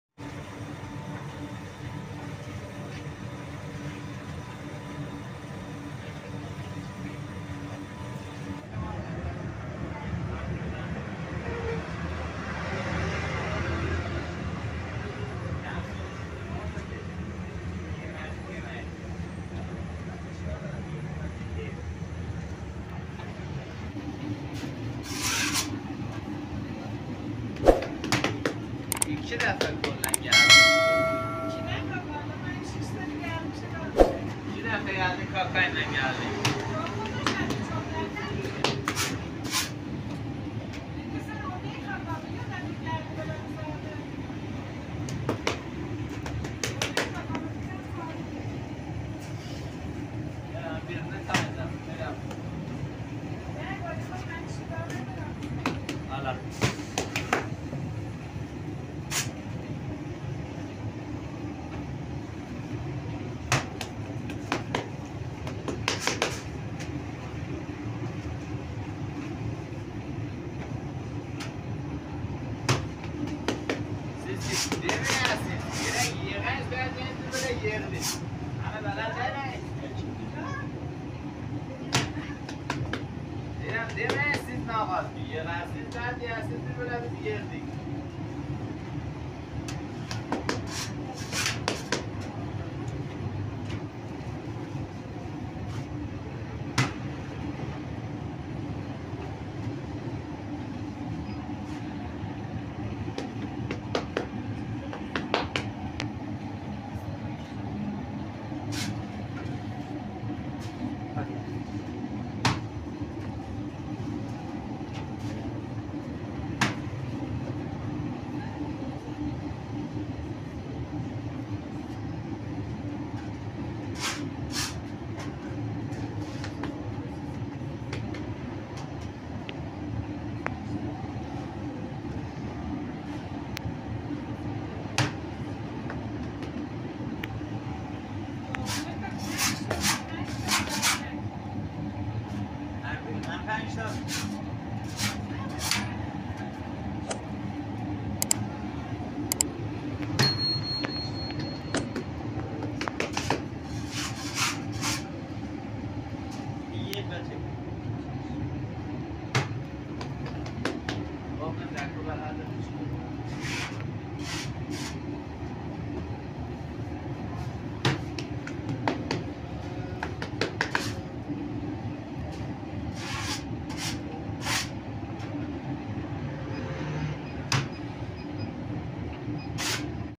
Local Iranian bread | Baked in the TANDOOR | popular types of Bread
breadoont
Hello, I am a documentary maker who makes a documentary about the bread baking method of different countries.